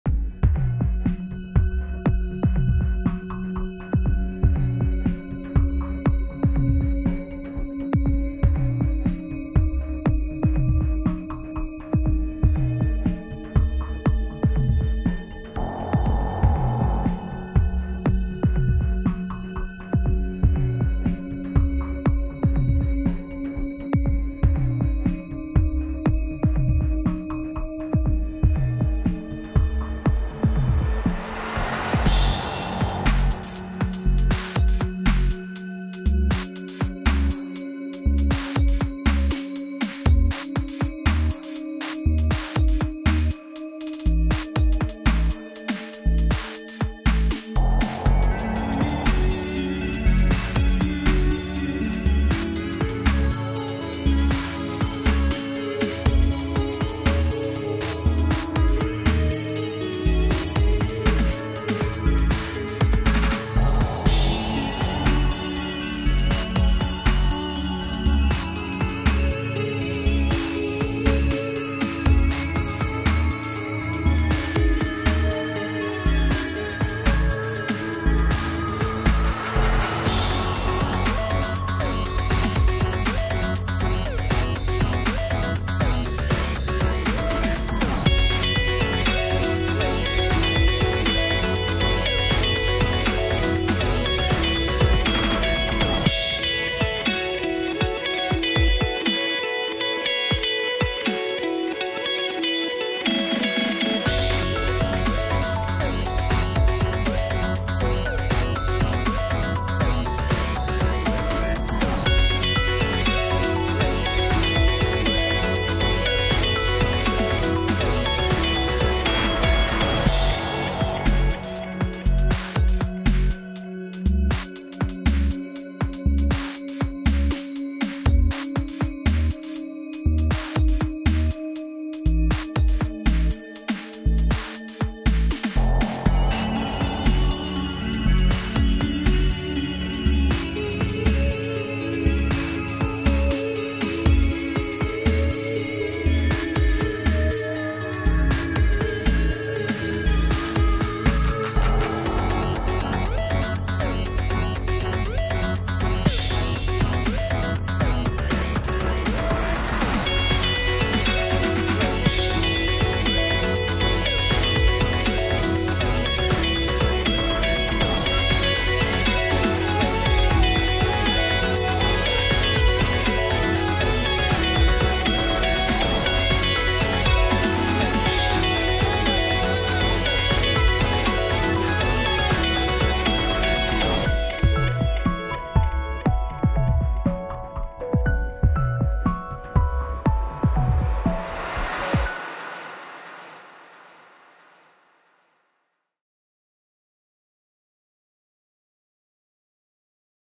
Will you welcome our AI overlords? | Ai art | 4k
AI-Generated art accompanied by original music by AIGent.